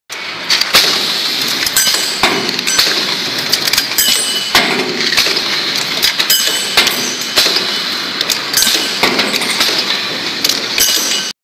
Máy cắt ống inox mỏng, ống lớn không móp, không ba via, cháy LH
máy cắt ống inox công nghệ mới không dùng lưỡi, không tiếng ồn, nhanh, chính xác, không móp méo, không ba via..